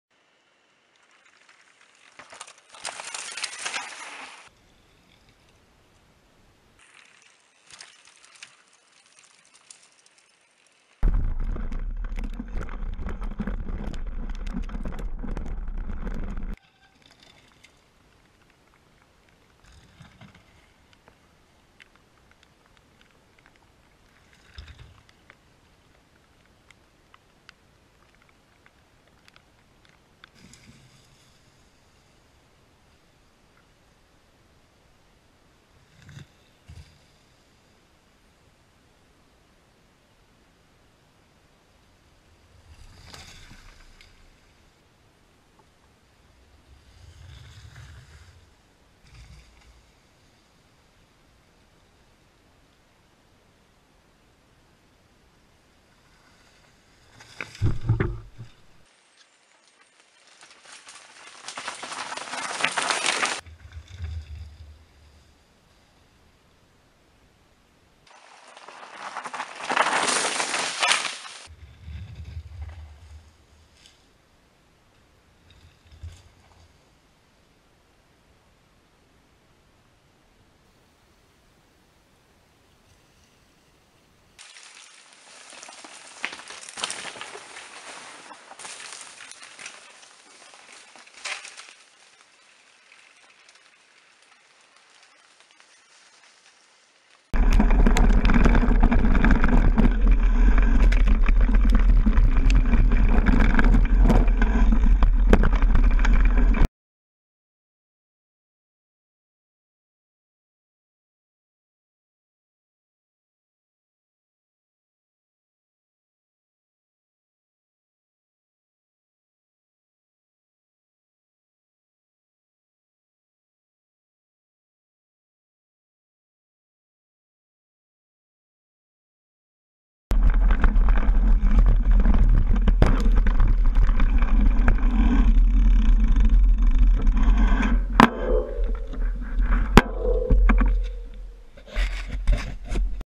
VTT DH Giant reign X1
Dh en pays de sault avec mon enduro Giant Reign X1
Video fait a la vavite acose de la pluie